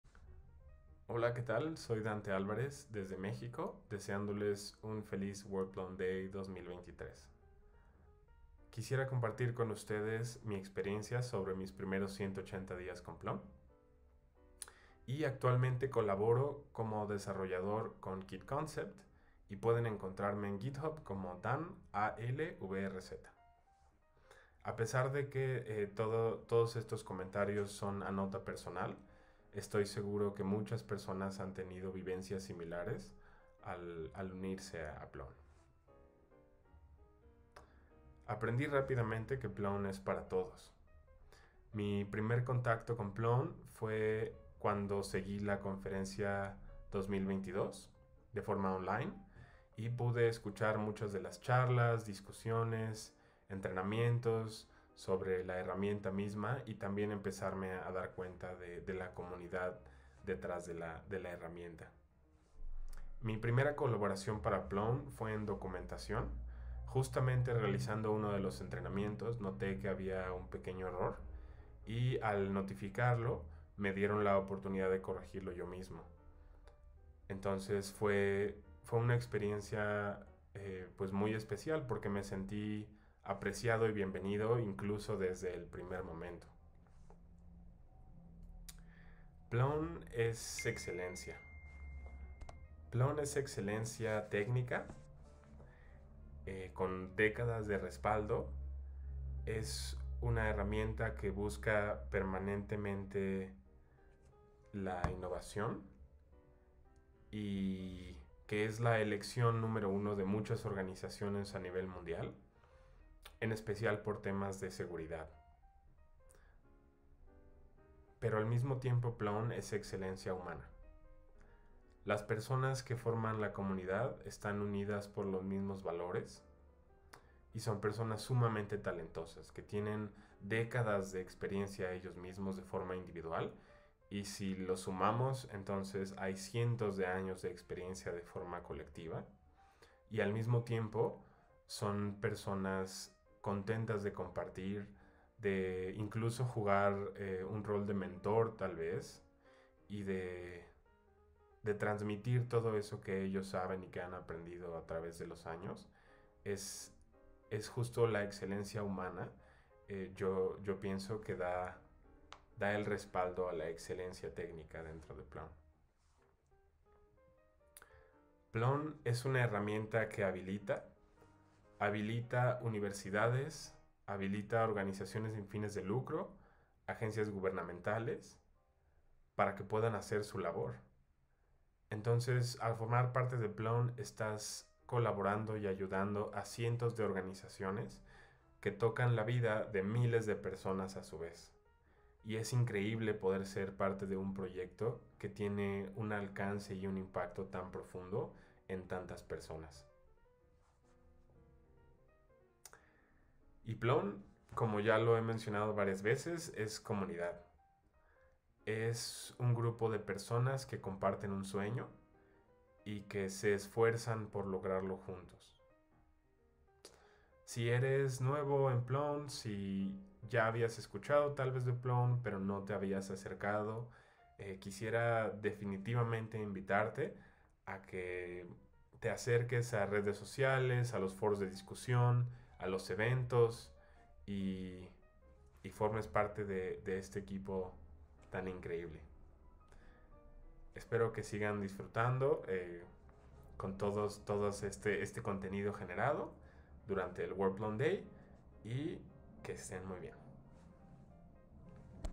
180 Días con Plone
Dante Álvarez comparte su experiencia de 180 días usando Plone